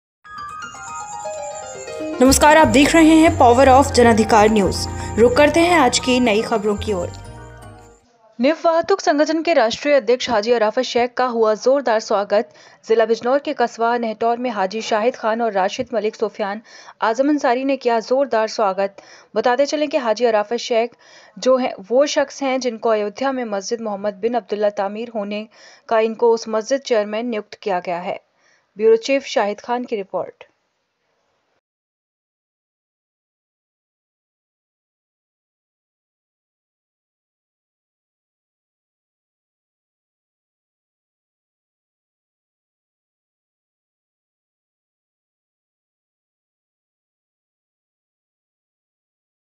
बिजनौर#मस्जिद मोहम्मद बिन अब्दुल्ला तामीर होने इनको मस्जिद चेयरमैन नियुक्त किया है
निव वाहतुक संगठन के राष्ट्रीय अध्यक्ष हाजी अराफात शैक  का हूवा जोर दार स्वागत किया गया जिला बिजनौर के कस्बा नेहटौर में  हाजी शाहिद खान और राशिद मालिक सुफियान आज़म अंसारी ने किया जोर दार स्वागत यह बताते चलें कि हाजी अराफात शैक  जो है ये वोह शख्स हे जिनको अयोध्या मैं मस्जिद मोहम्मद बिन अब्दुल्ला तामीर होने इनको उस मस्जिद चेयरमैन नियुक्त किया है ब्यूरो चीफ शाहिद खान की रिपोर्ट